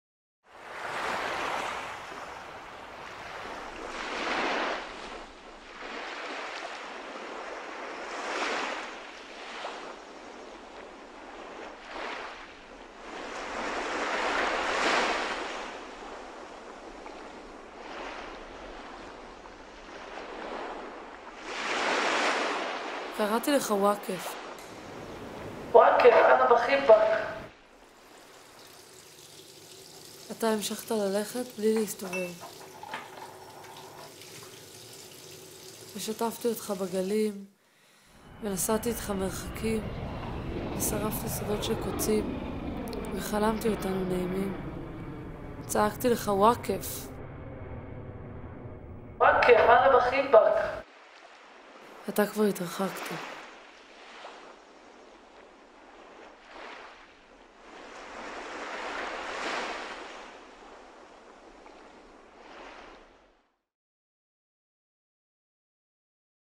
Wakef | תהילה חכימי
מתוך ספר השירים הראשון של תהילה חכימי ׳מחר נעבוד׳
הספר בעריכת עודד וולקשטיין ובהוצאת ׳טנג׳יר׳

וידאו: ענבל מעוז, בימוי צילום ועריכה.
תודה מיוחדת: אפרת חכימי, ׳אופנת אורטל׳, כרמן אלמקייס עמוס ו׳בית אחותי׳, אופיר טובול ואלון בר.
הפקה בים: שלומי חתוכה וארז יוסף